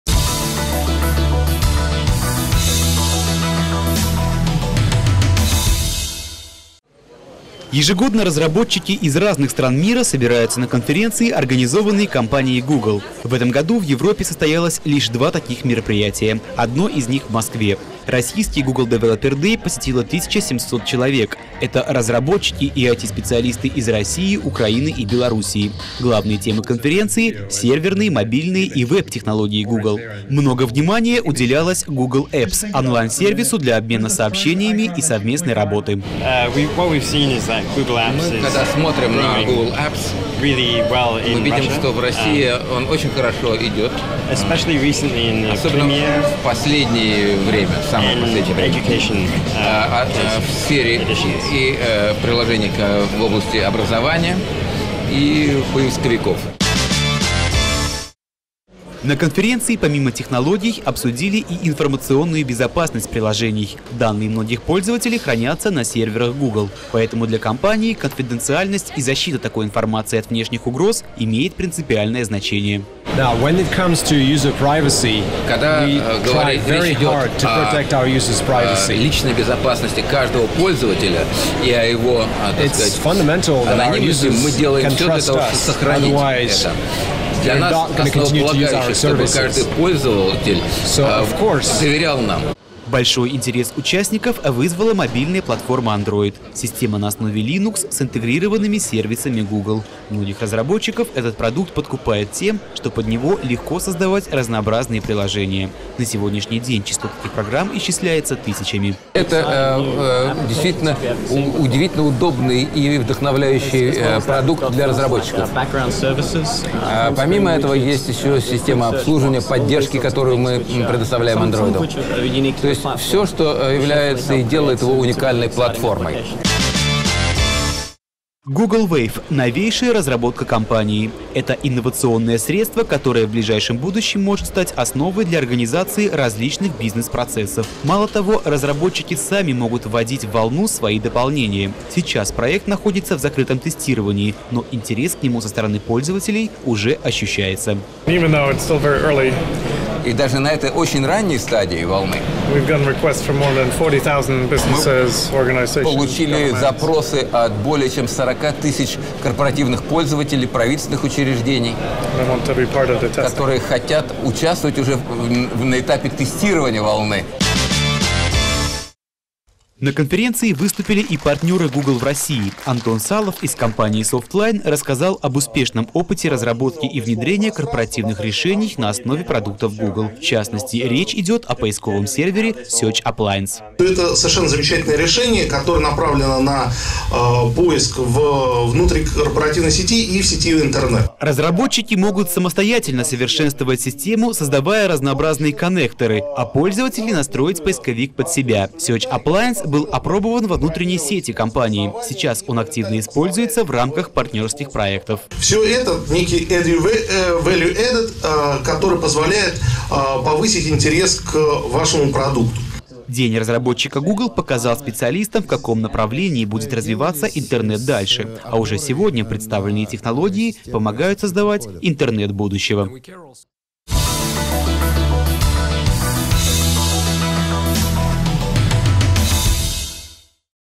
Google Developer Day в Москве
В Москве состоялась конференция для программистов и разработчиков веб-приложений Google Developer Day 2009. Ведущие инженеры компании рассказали, как с помощью продуктов Google создавать социальные, мобильные и географические приложения.